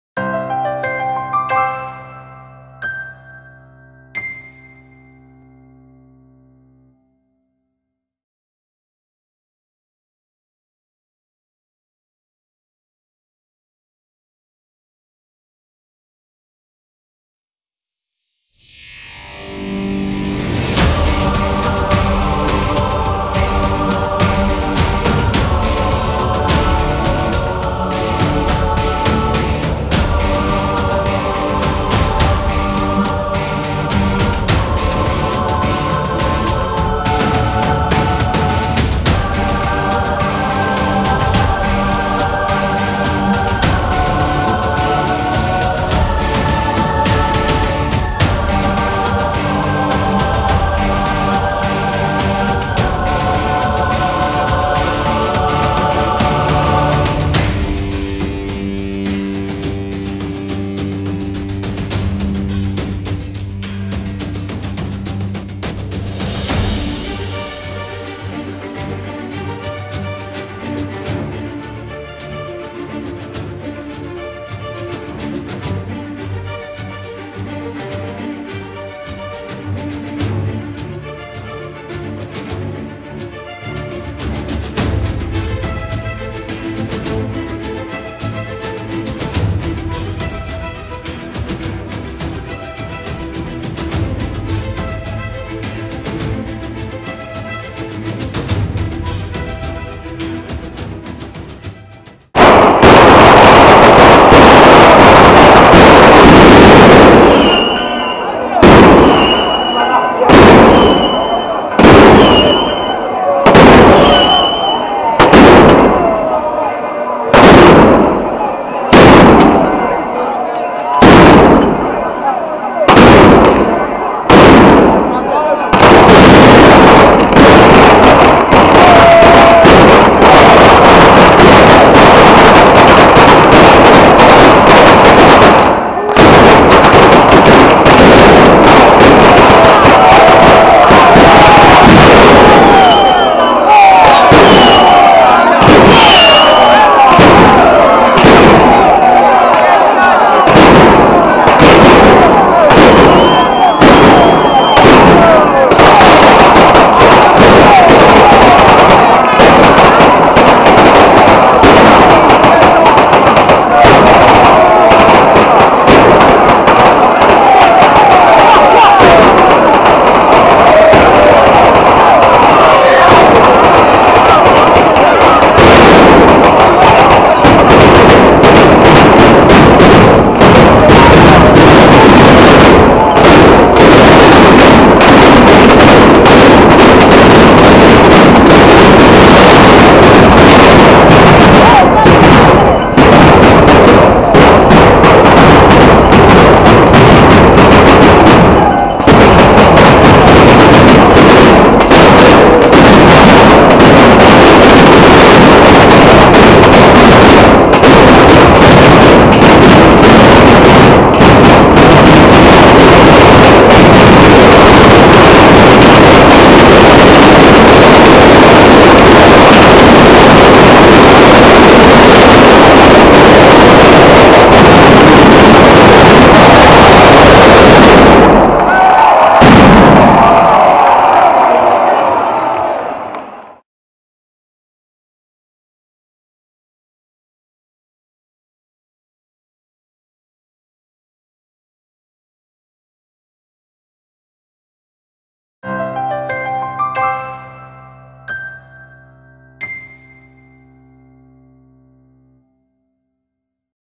Festa del Soccorso 2016 - San Severo (FG) - batteria Piazza Carmine 16 maggio
Festa del Soccorso 2016 - San Severo (FG) - batteria Piazza Carmine 16 maggio eseguita dalla dittà Pirogiochi di Chiarappa Giuseppe e Figlio da San Severo (FG).